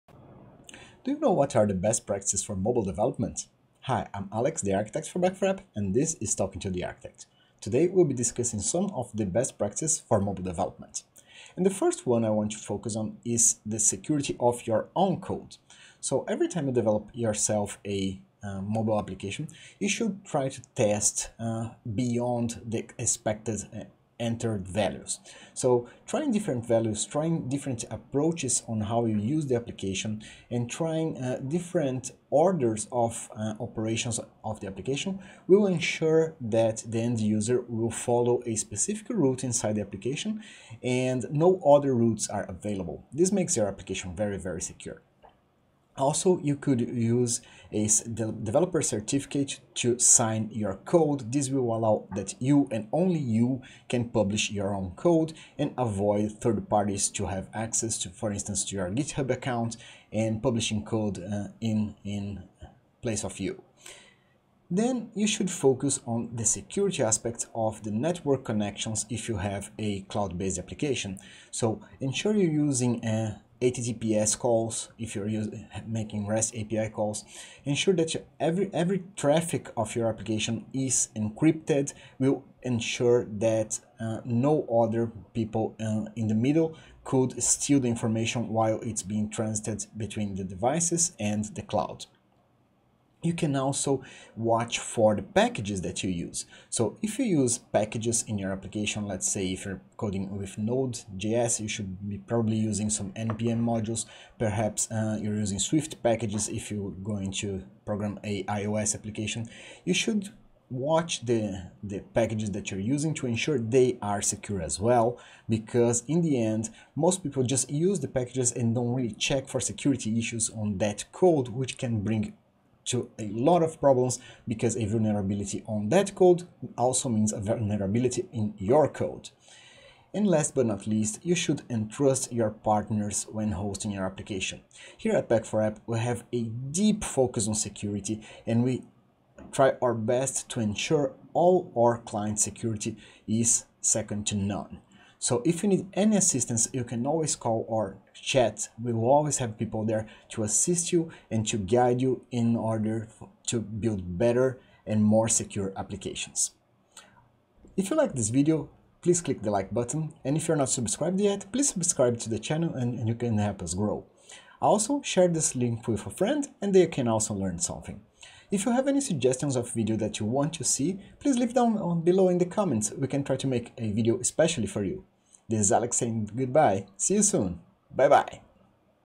Mobile Development Best Practices
Today in Talking to the Architect, we will discuss some Mobile Development Best Practices.